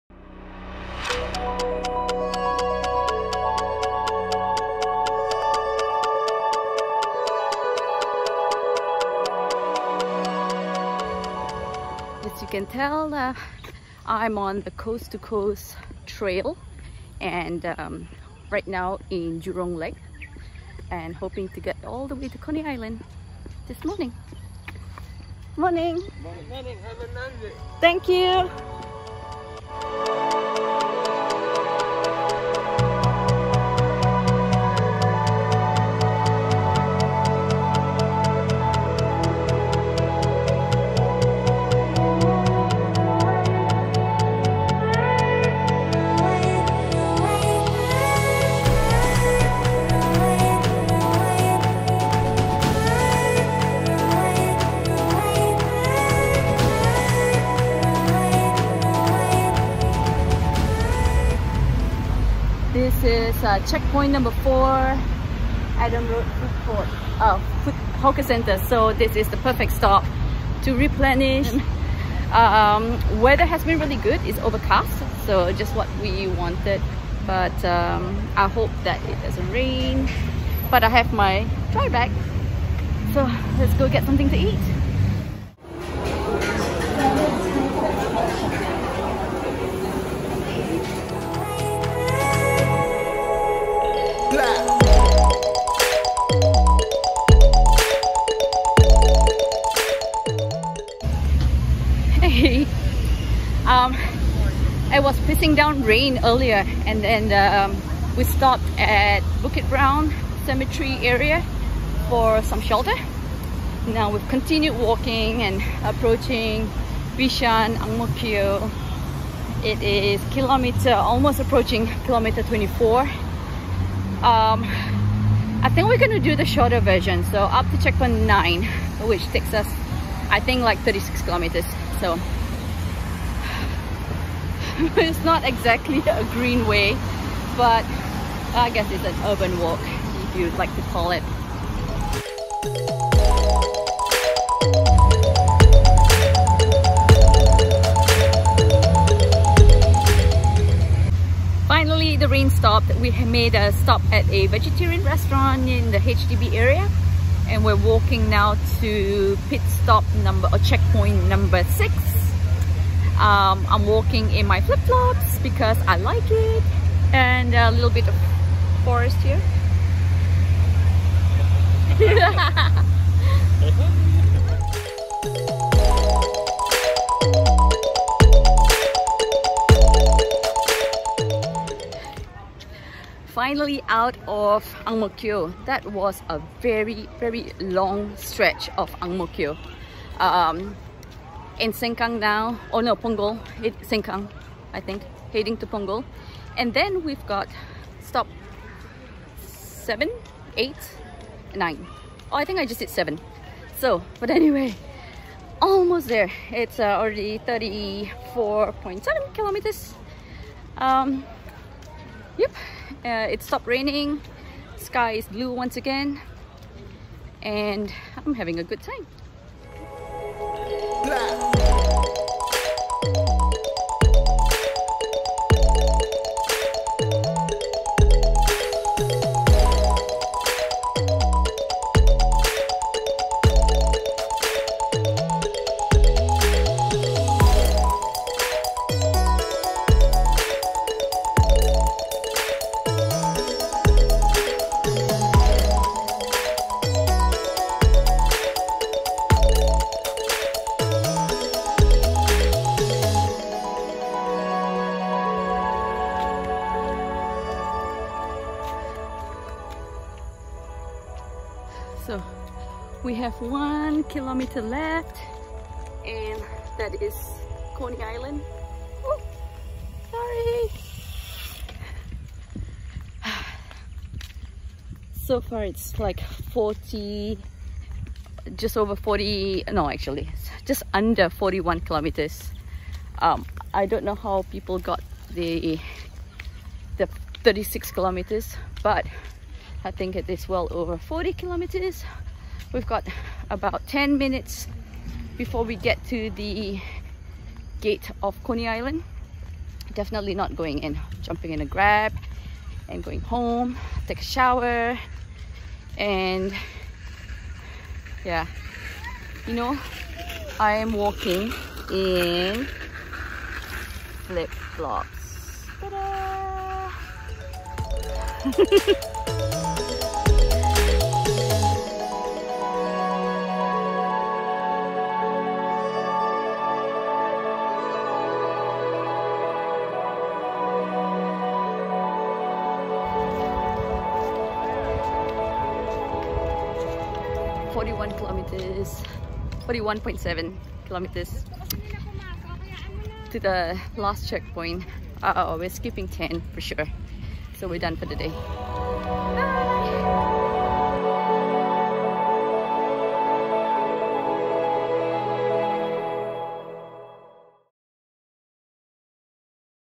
Walking across Singapore , Coast to Coast C2C
This trail spans across a good portion of the island. There are Park Connectors linking Jurong Lake Gardens in the west all the way to Coney Island Park in the northeast.
It was a little too urban to be called a hike for my linking but I am glad I finally found a group of people willing to do it with me.

Musician : ASHUTOSH